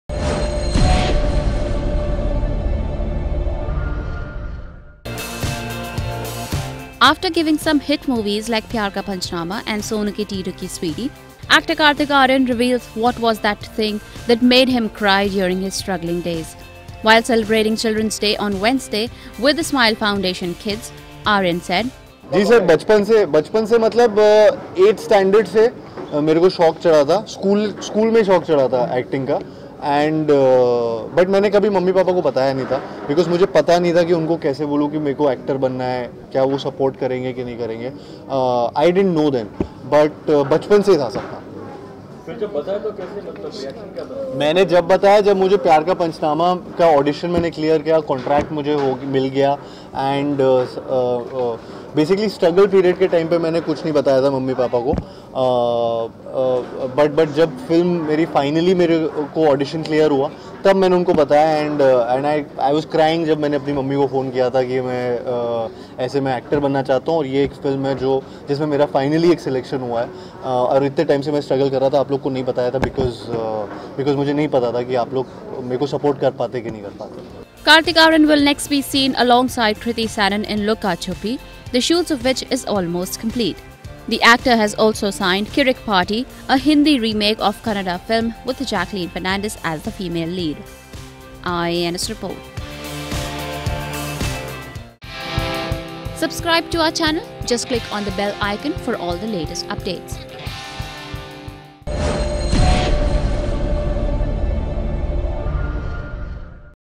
What made Kartik Aaryan CRY
After giving some hit movies like 'Pyaar Ka Punchnama' and 'Sonu Ke Titu Ki Sweety', actor Kartik Aaryan reveals what was that thing that made him cry during his struggling days. Kartik Aaryan will next be seen alongside Kriti Sanon in 'Lukka Chuppi', the shoots of which is almost complete.